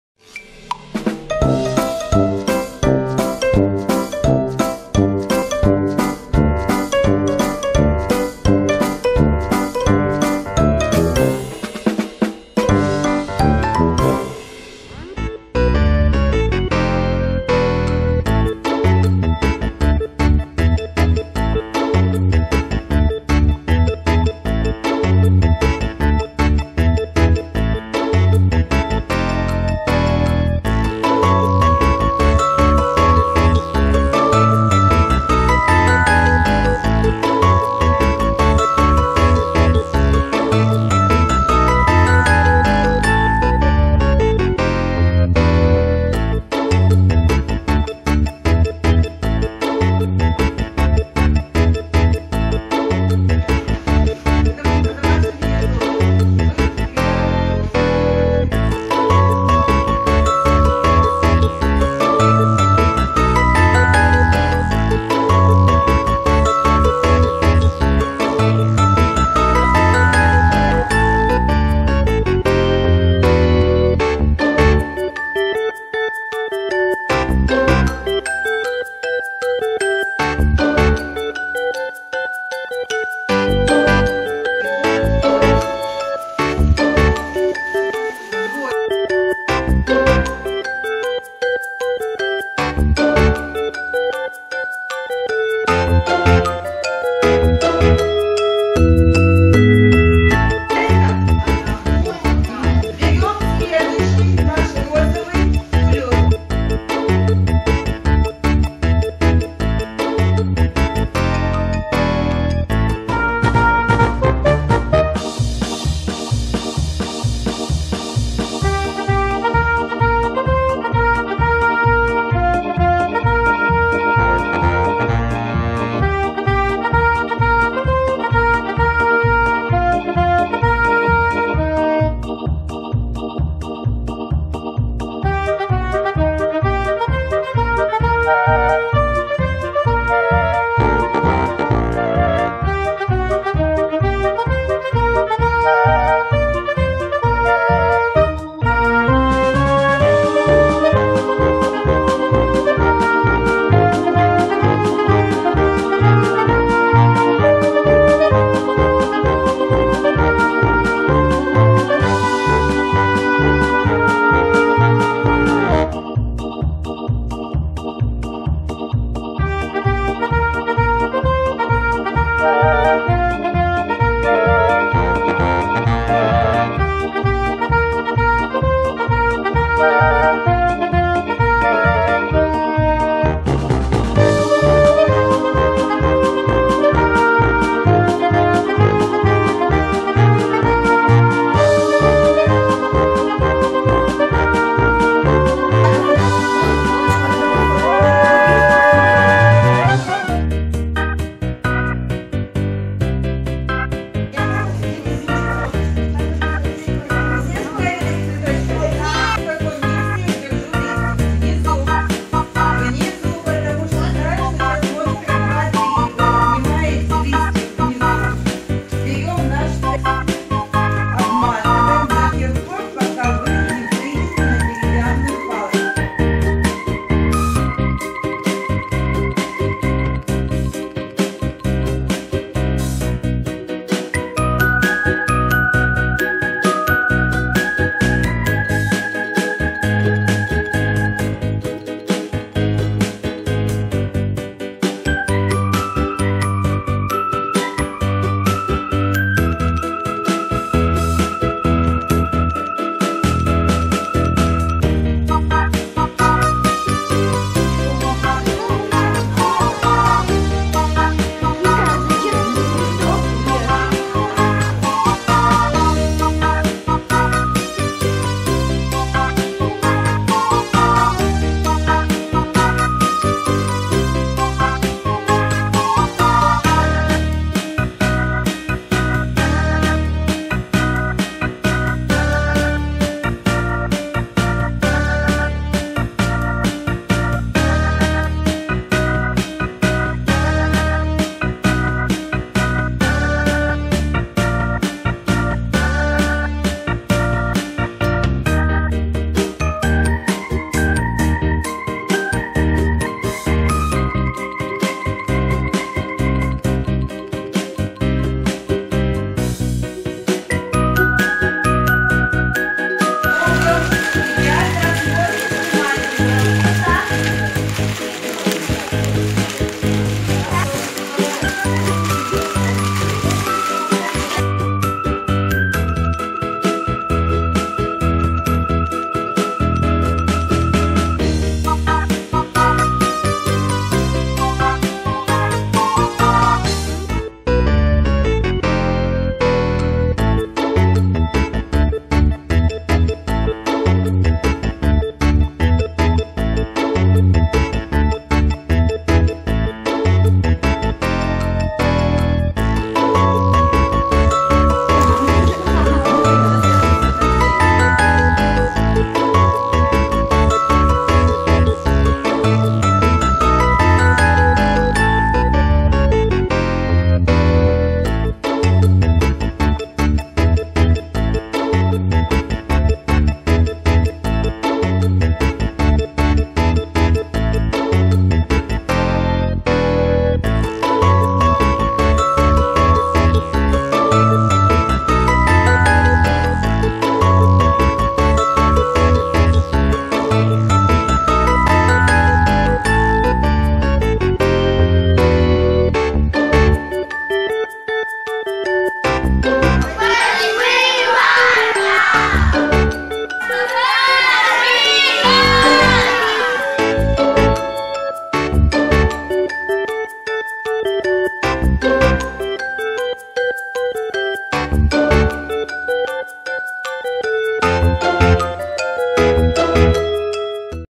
Мастер-класс Тюльпан из гофрированной бумаги с конфетой внутри в 3-Б классе.
Мастер класс#Тюльпан#из гофрированной бумаги
Мастер-класс Тюльпан из гофрированной бумаги с конфетой внутри к 8 Марта в 3-Б классе.